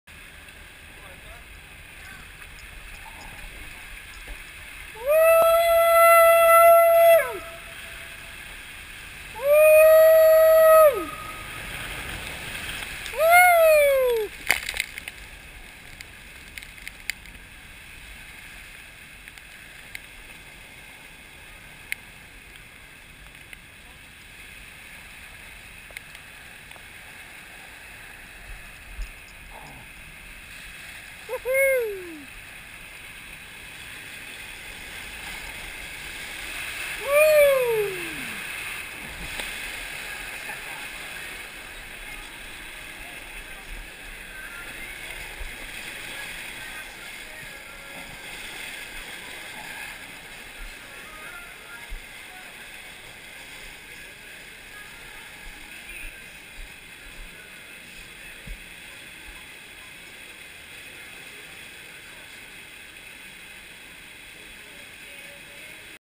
Norwegian Epic tube slide
This slide is a blast! I must have went down it 40 times!